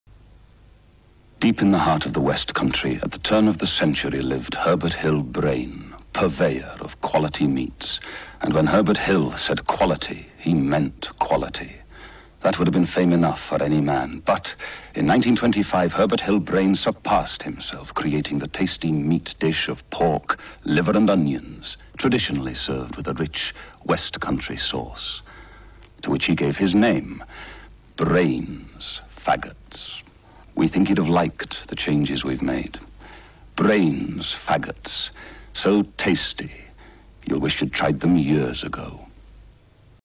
Brains' Faggots advert - 1984
In a spot of moonlighting, James Bond 007 (Timothy Dalton) earns a bit of extra cash by extolling the virtues of the traditional west-country meatballs in full-on heavy-pronunciation ACTOR mode, much to the amusement of easily er.. amused schoolboys across the land.